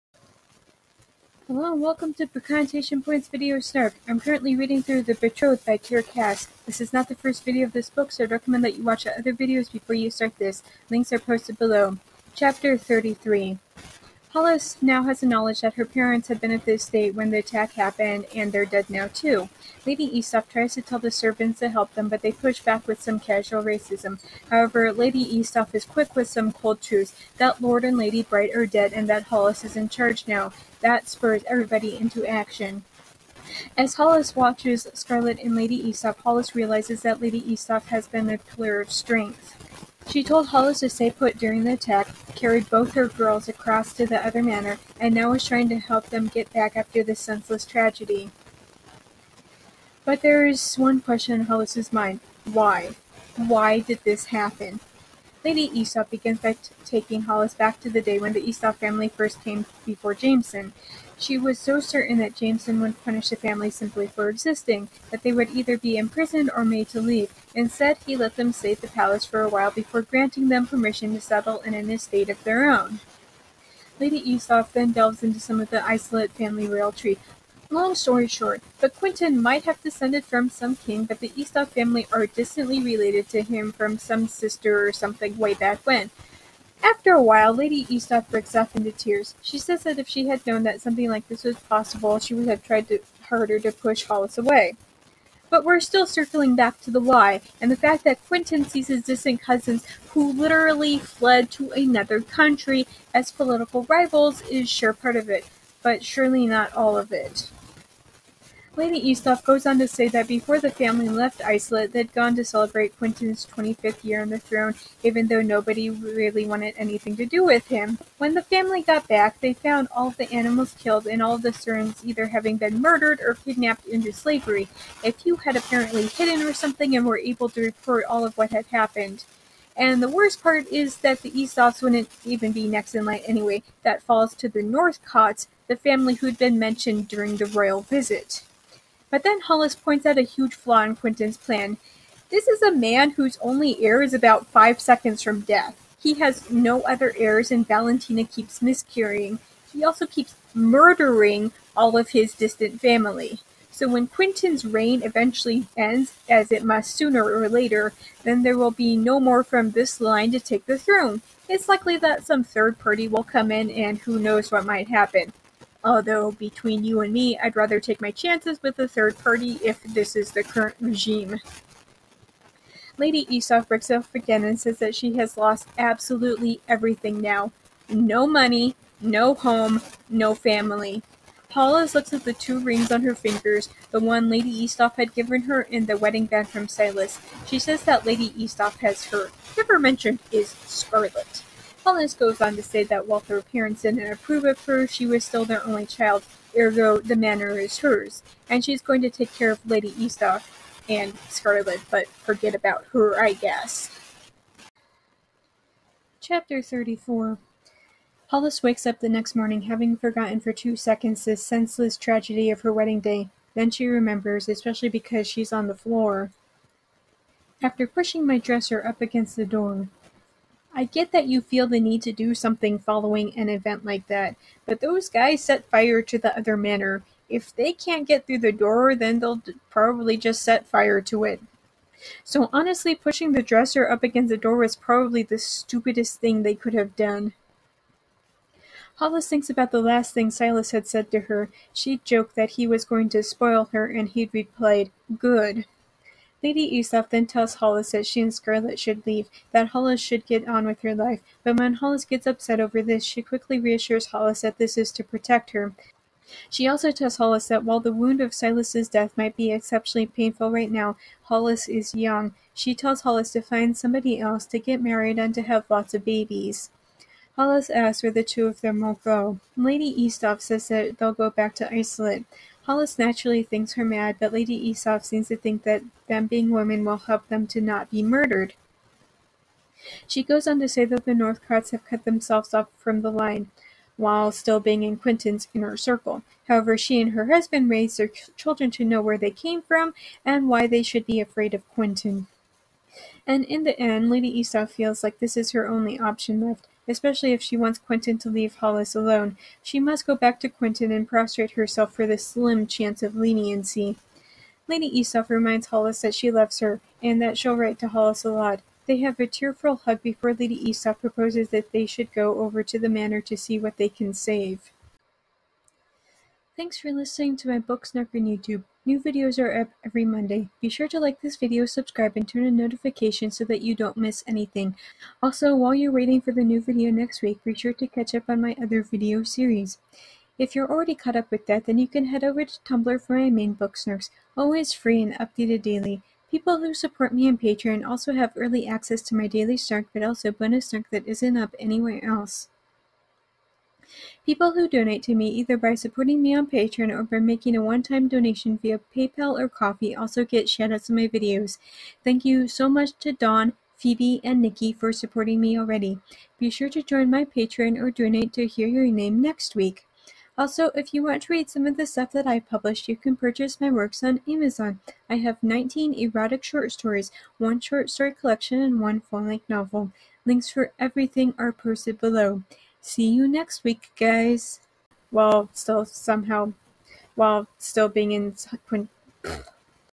Book Snark: The Betrothed, chapters 33 & 34
Sarcastic book commentary of The Betrothed by Kiera Cass. Now with built-in subtitles for the hard-of-hearing community, those who don't speak English well, or those who just have problems with comprehension!

In the mean time, check out my tumblr for more snark